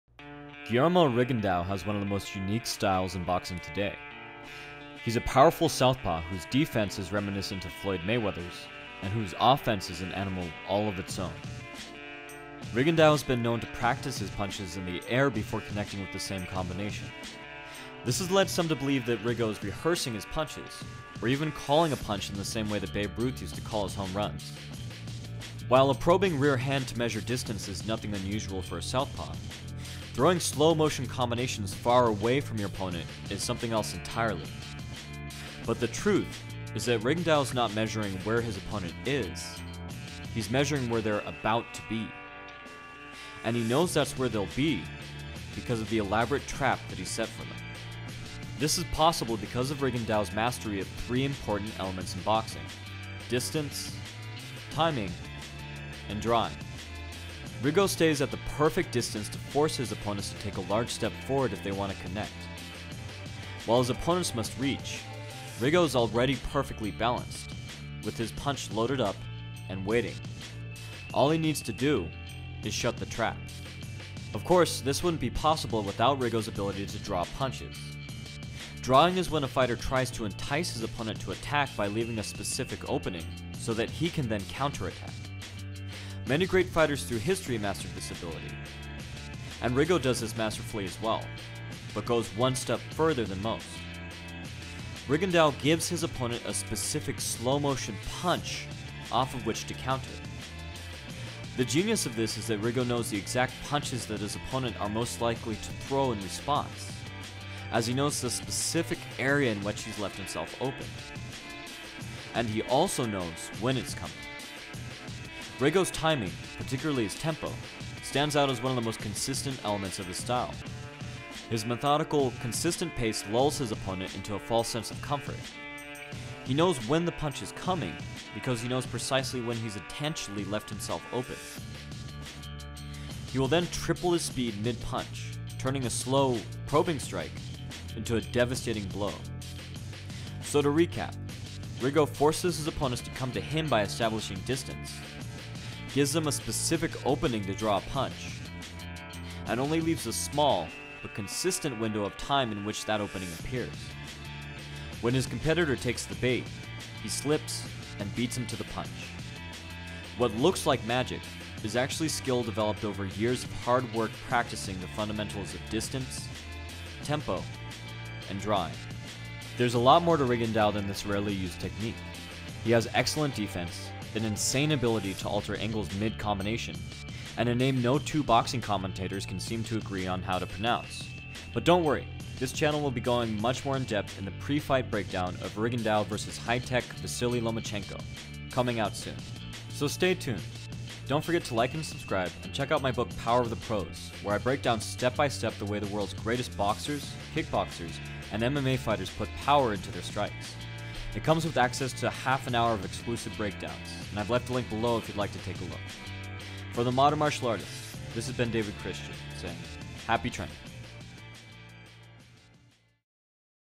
Rigondeaux's Mid-Fight Shadowboxing & Counter Punching Explained - Technique Breakdown
Guillermo Rigondeaux has one of the most unique styles in boxing today. He’s a powerful southpaw set to face off against Vasyl Lomachenko. His defense is reminiscent of Floyd Mayweather’s, and his offense is an animal all of it’s own.

Rigo has been known to practice his punches in the air before connecting with the same combination. This has led some to believe that Rigo is rehearsing his punches, or even calling out a punch in the same way that Babe Ruth used to call out his home runs.

While a probing rear hand to measure distance is nothing unusual for a southpaw, throwing slow motion combinations far away from your opponent is something else entirely. But the truth is that Rigo is not measuring where his opponent is, he is measuring where they are about to be…

And he knows that’s where they’ll be, because of the elaborate trap that he’s set for them.

This is possible because of Rigo’s mastery of three important elements in boxing. Distance, timing, and drawing.